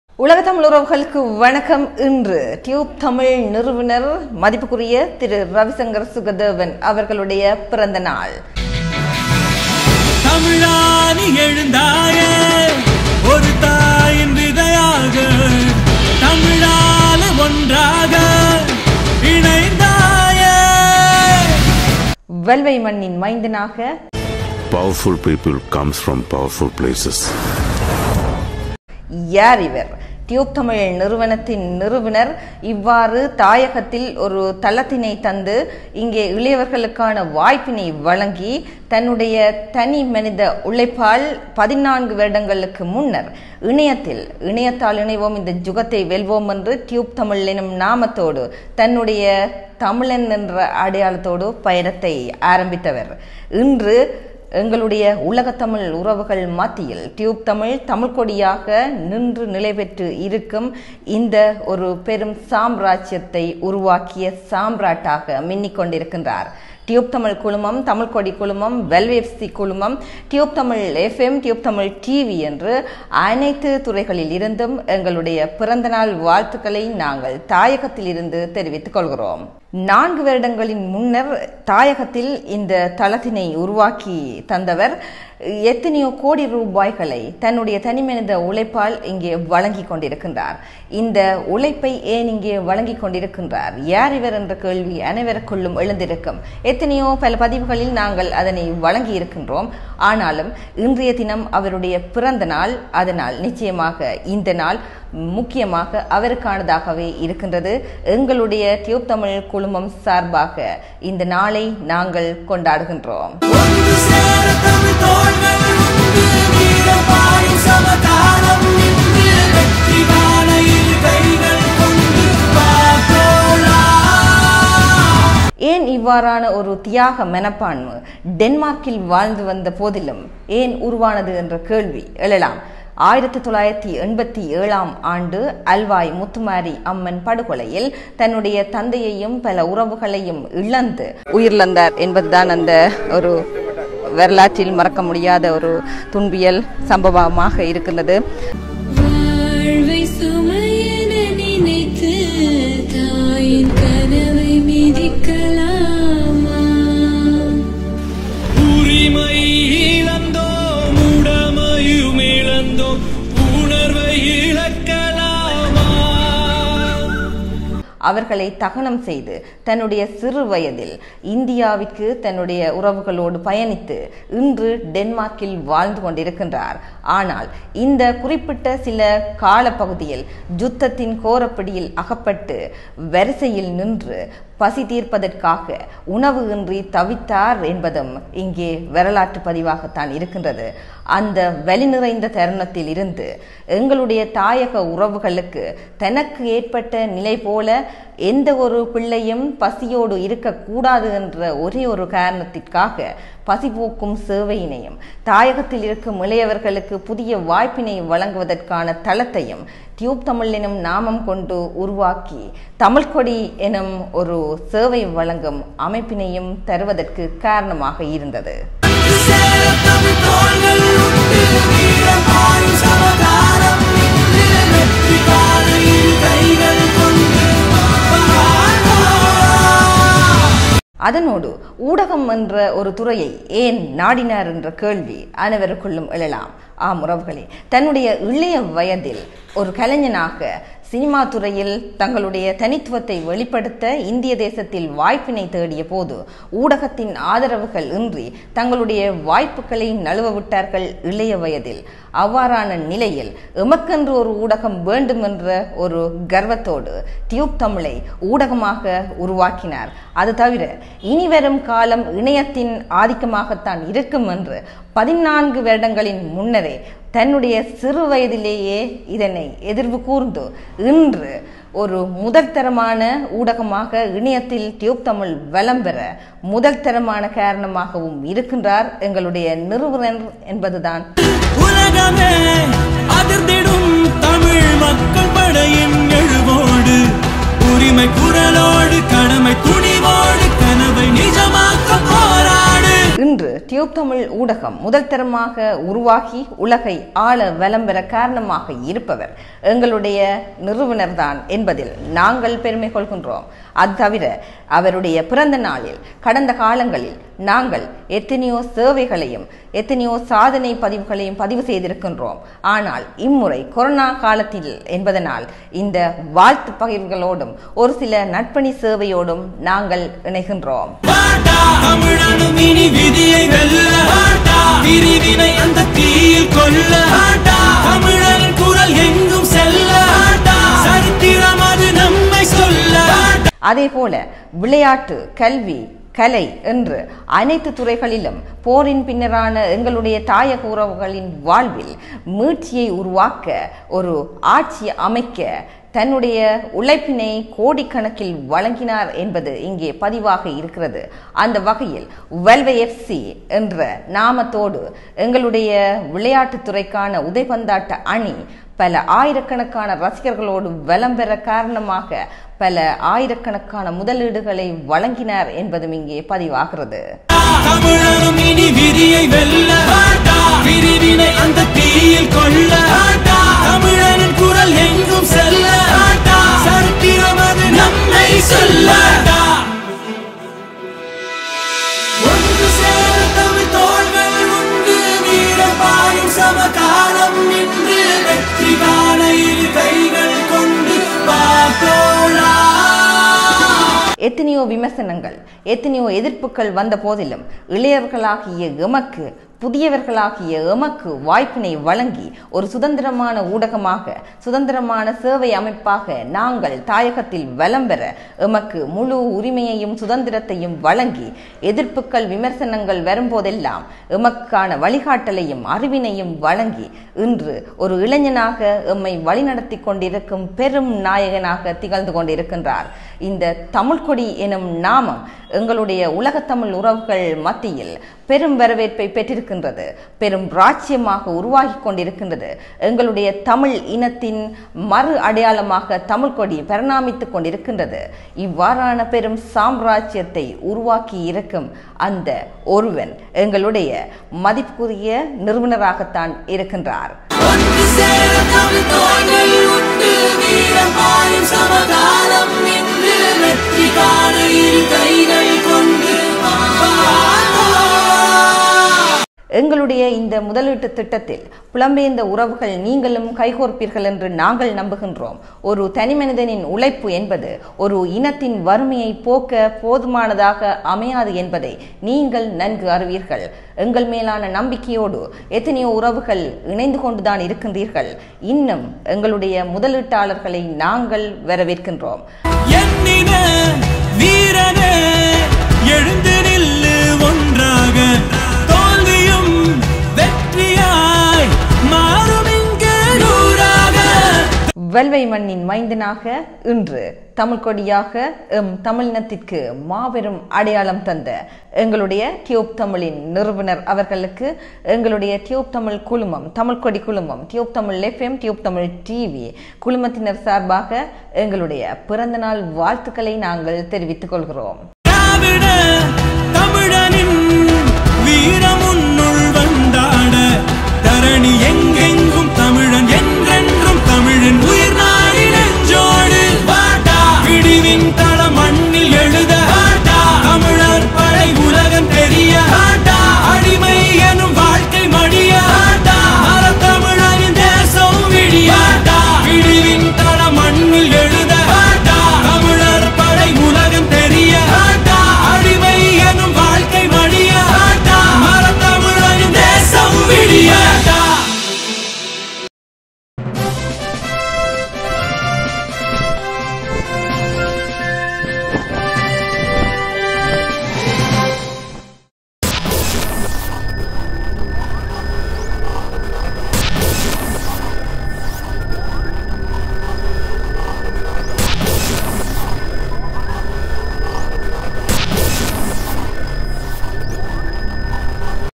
தமிழன் என்று தலை நிமிர்ந்த ரியூப்தமிழ் நிறுவுனர் ரவிசங்கர் சுகதேவனுக்கு பிறந்தநாள் நல்வாழ்த்துக்கள்
தமிழன் என்று தலை நிமிர்ந்த ரியூப் தமிழ் நிறுவுனர் மதிப்பிற்குரிய திரு.ரவிசங்கர் சுகதேவனுக்கு எம் இனிய பிறந்தநாள் நல்வாழ்த்துக்கள்.